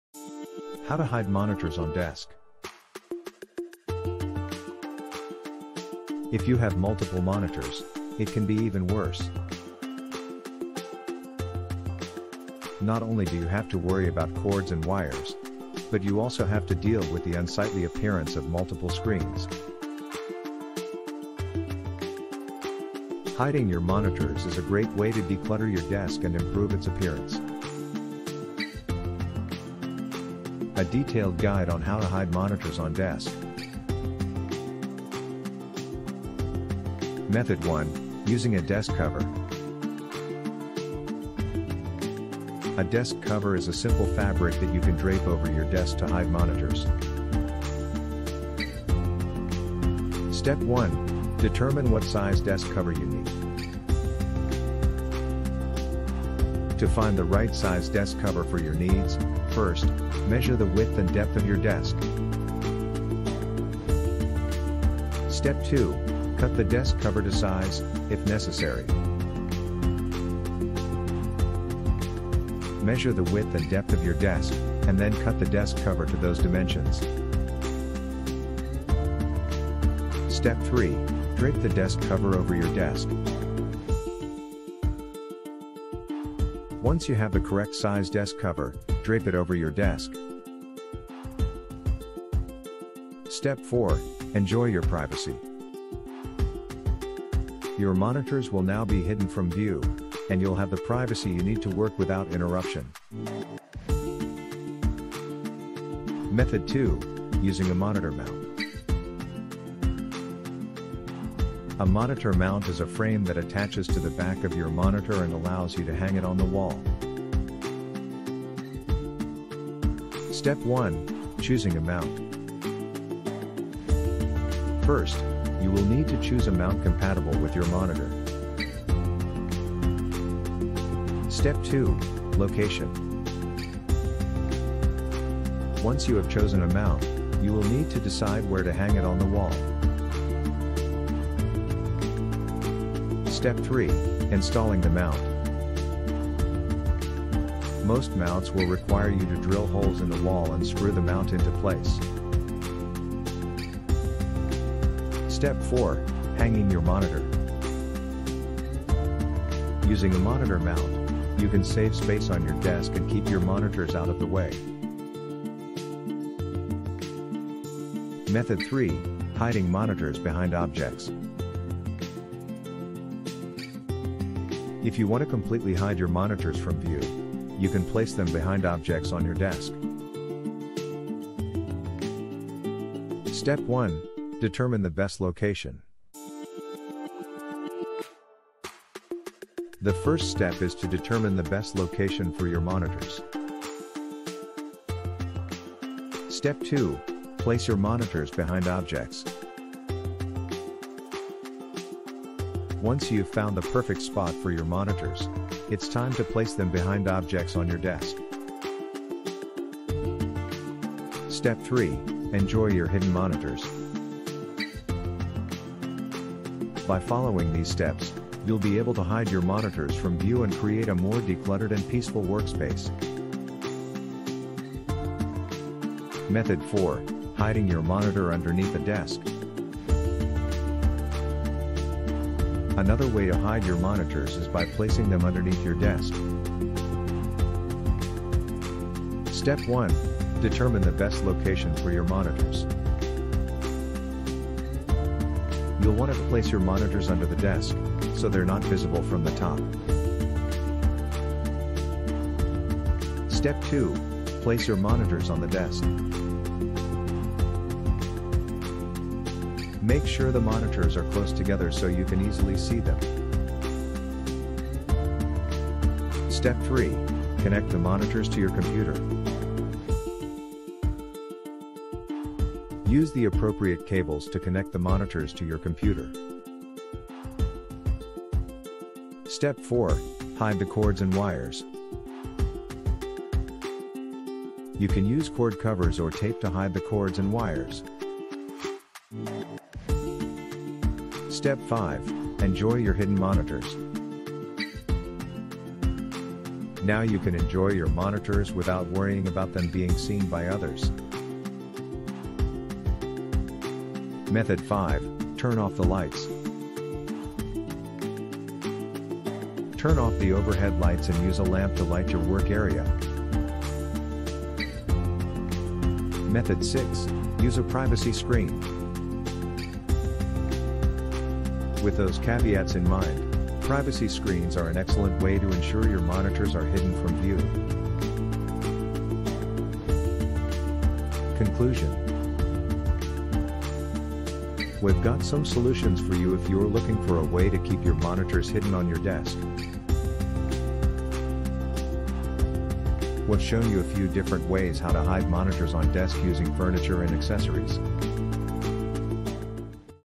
How to Hide Monitors on Desk
If you’re like me, you have a lot of electronics on your desk. Unfortunately, there’s little room left for anything else between your computer, monitor, printer, and other assorted gadgets. If you’re looking for a way to free up some space on your desk, consider hiding your monitors. It’s a simple solution that can make a big difference.

In this video, we’ll discuss how to hide monitors on desk. Keep reading to learn more. We all know how cluttered desktops can get. If you have multiple monitors, it can be even worse. Not only do you have to worry about cords and wires, but you also have to deal with the unsightly appearance of multiple screens.